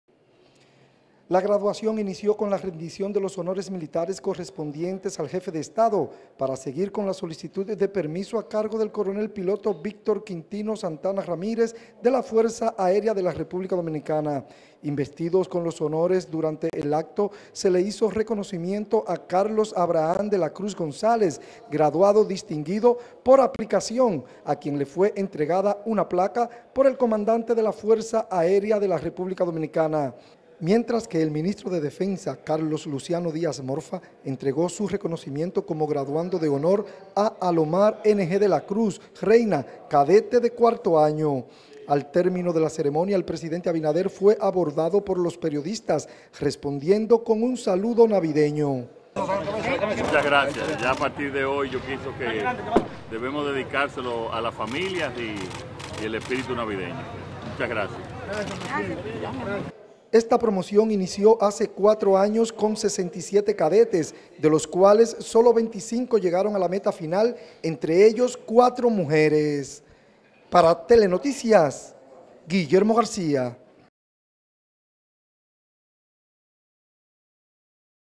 Abinader encabeza graduación de cadetes en FARD
El presidente Luis Abinader encabezó este miércoles  la graduación de 25 cadetes de la Fuerza Aérea de República Dominicana en la Vigésimo segunda promoción “Mayor general piloto Fernando Evangelista Cruz Méndez” de la Academia Aérea "General de Brigada Piloto Frank A. Féliz Miranda".